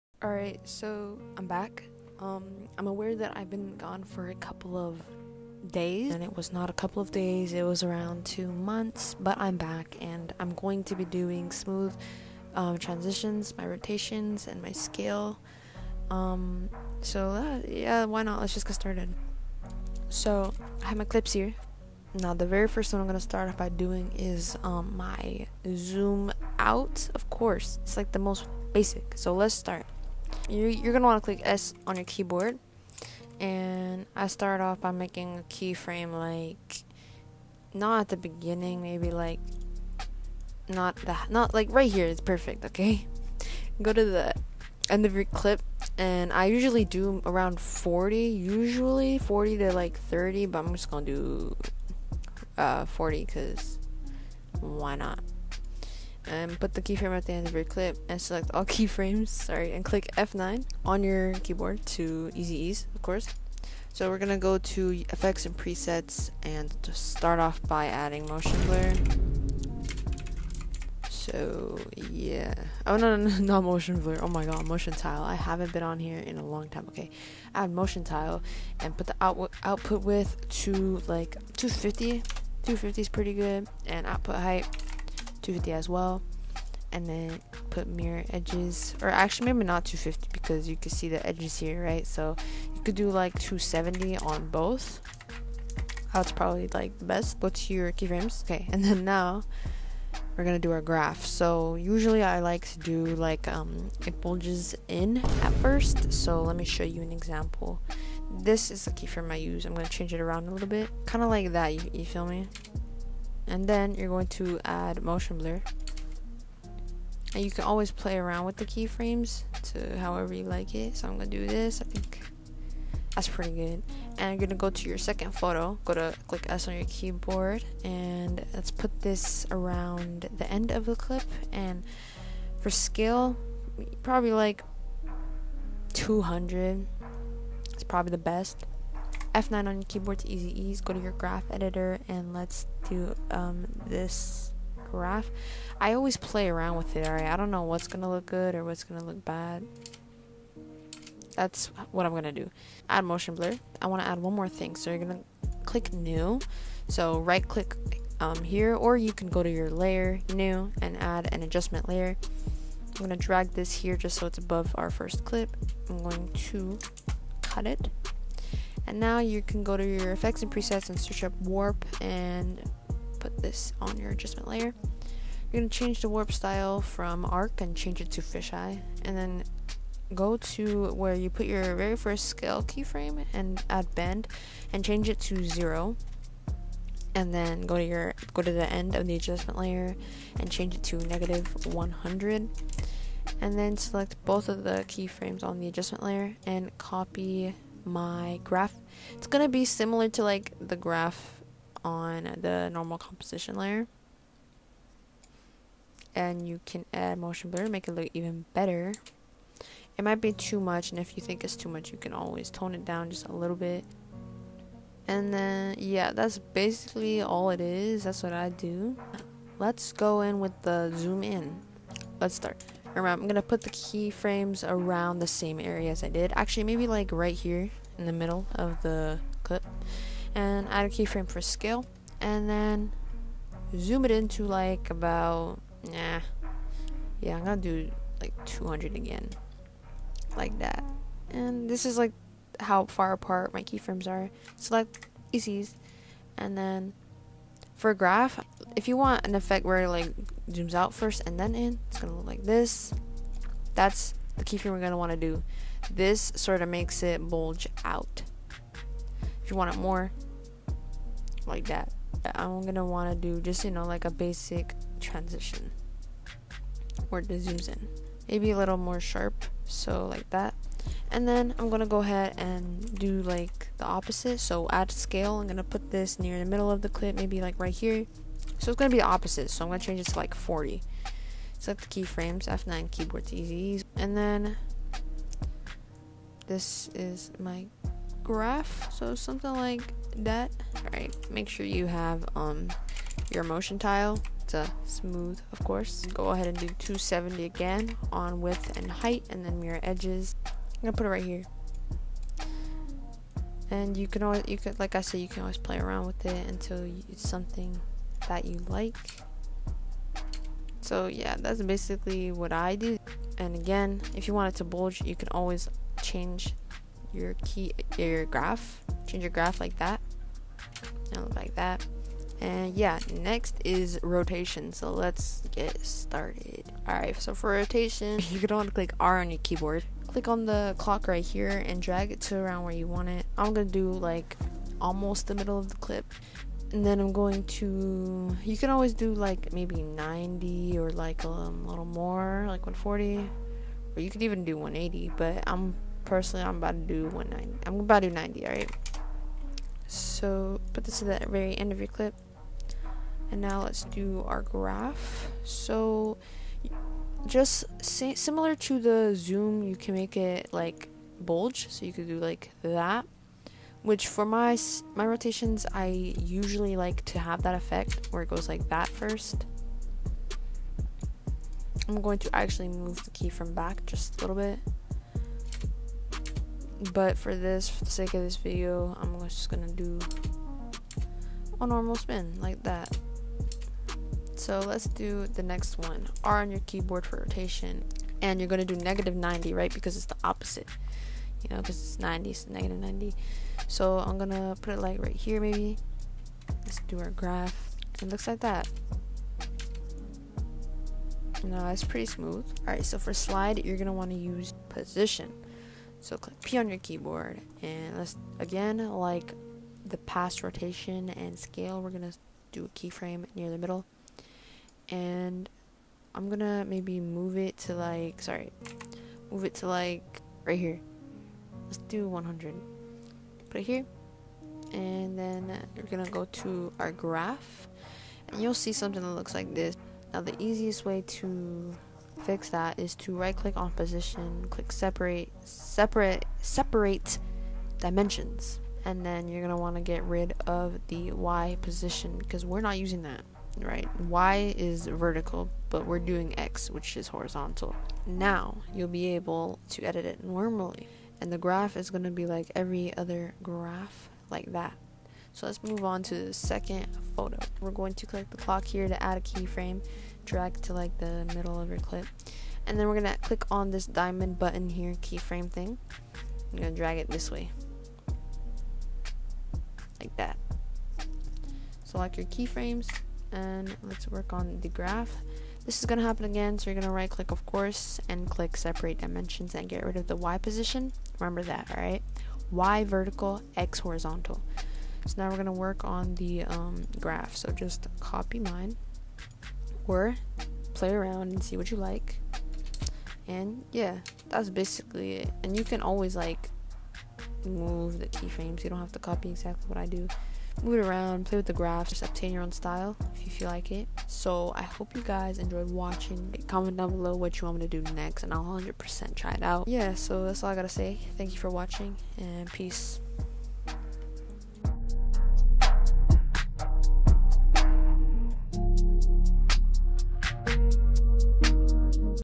smooth transitions || after effects tutorial
I'm back! And hopefully I'll be posting consistently

sub if this video helped you out

0:30 zoom out
3:42 zoom in
5:24 rotation
6:43 slide

wm remake: naevisvfx

this is my ONLY social media account i have made for ​⁠@hoser.